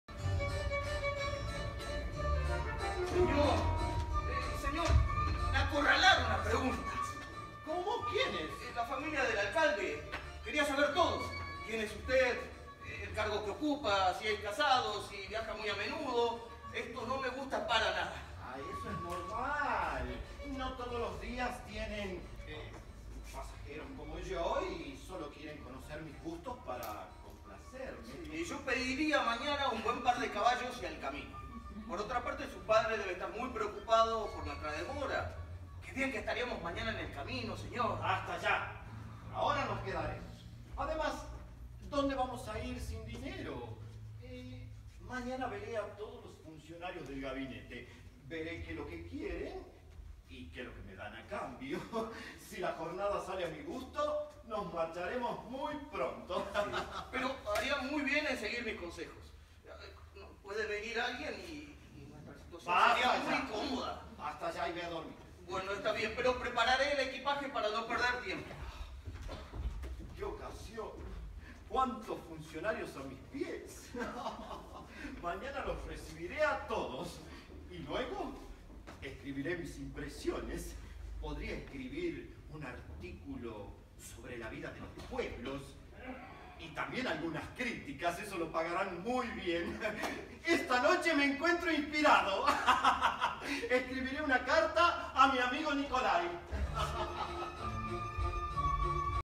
El Inspector - Parte 12
El criado comenta al inspector la interrogación de la familia del alcalde